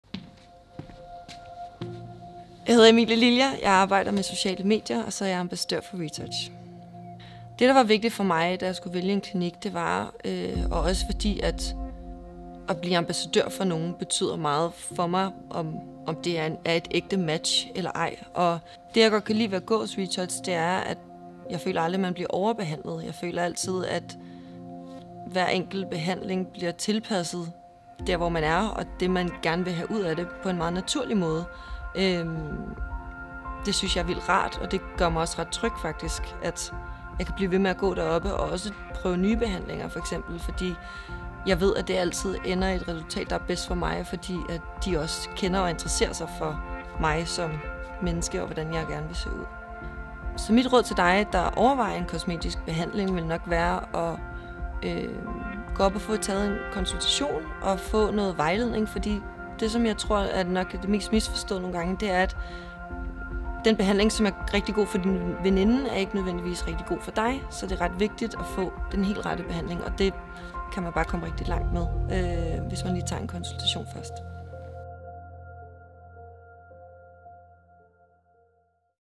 Emilie Lilja - Ambassadør for Retouch
Mød vores smukke ambassadør Emilie Lilja 🤍

Læs mere om Emilie Lilja og hvilke behandlinger, som hun får hos Retouch på vores hjemmeside